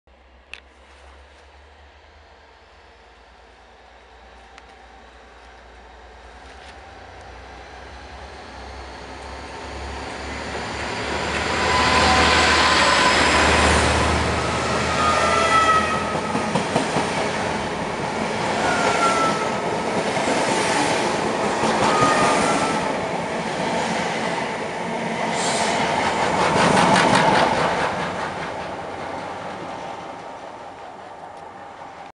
(HD) 57605 + Night Riviera Sleeper - St James' Park
First Great Western Class 57 No. 57605 'Totnes Castle' is seen passing St James' Park while leading 1A40 the 21:15 Penzance to London Paddington sleeper service in the early hours of Monday 5th April 2014.